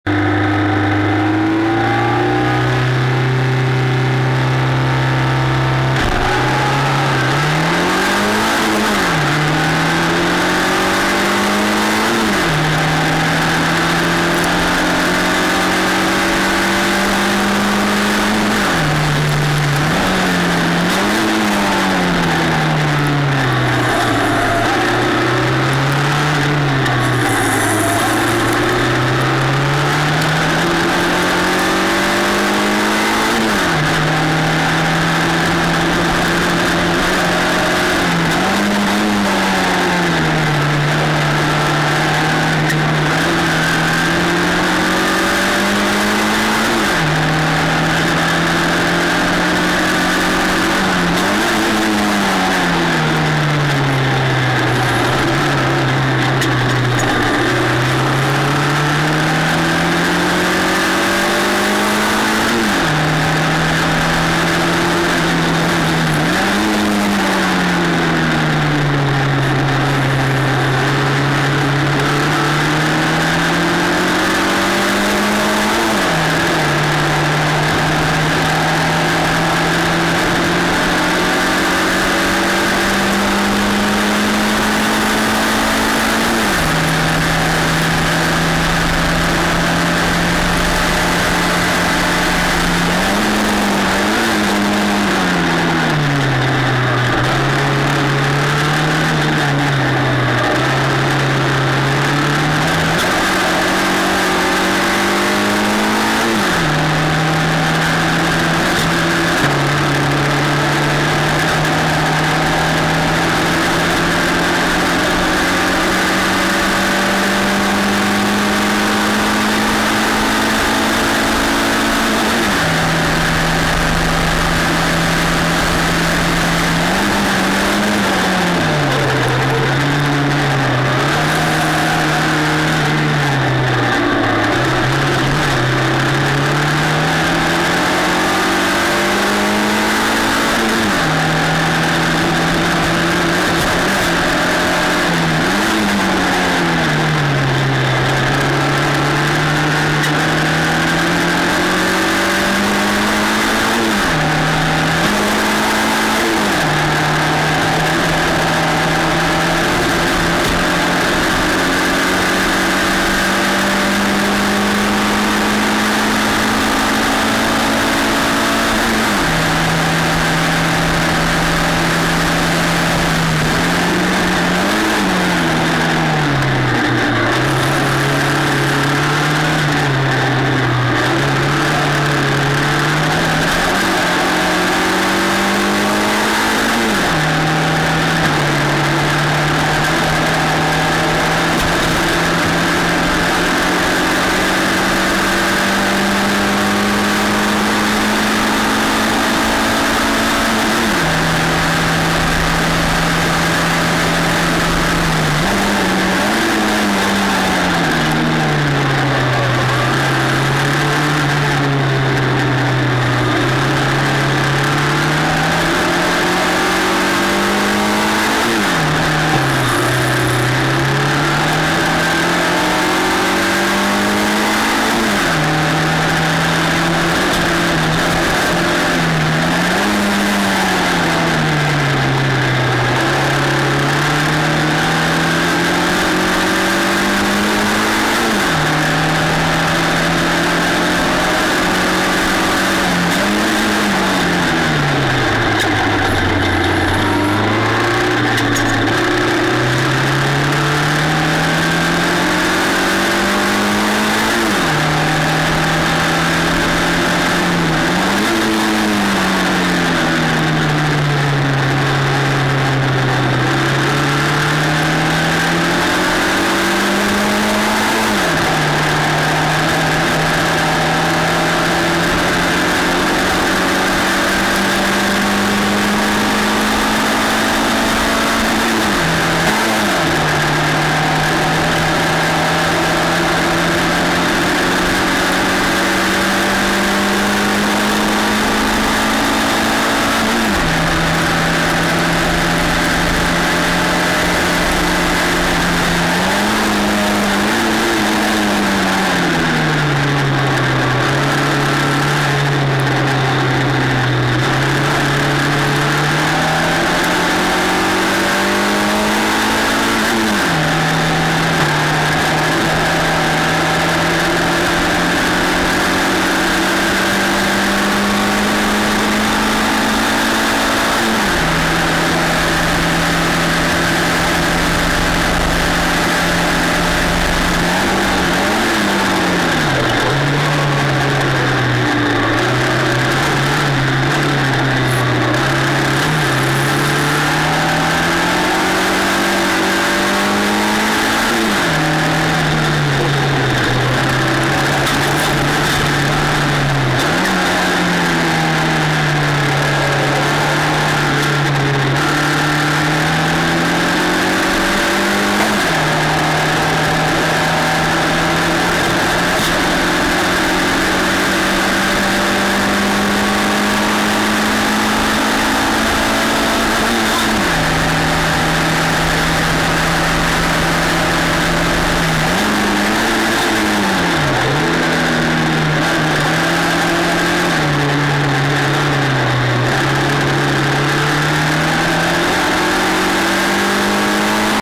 FLGB F4 2014
My First Year In The Formula FLGB F4 Category With Team Msport Back In 2014. It Is A Category Which Always Gives Us One Of The Best Wheel To Wheel Action.